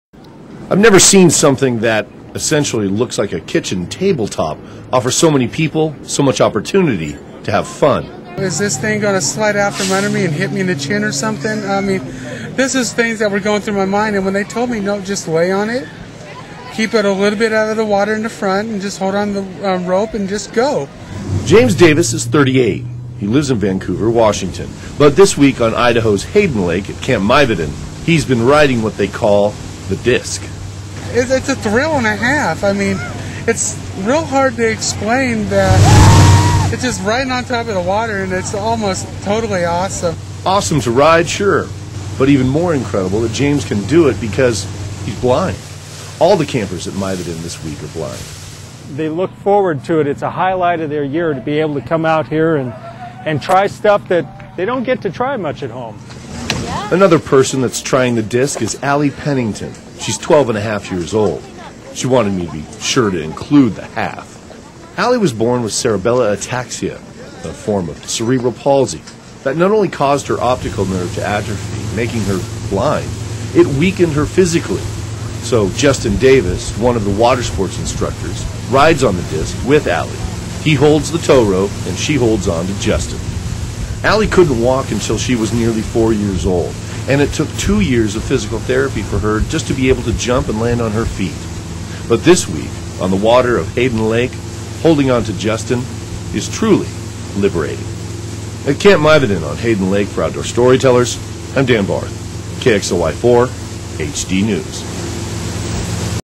Outdoor Storytellers - Ski Disc
On the surface, it looks like a round piece of plywood.  But for the blind campers at Camp Mivoden, it's a source of endless water fun, as Dan Barth explains in this edition of Outdoor Storytellers.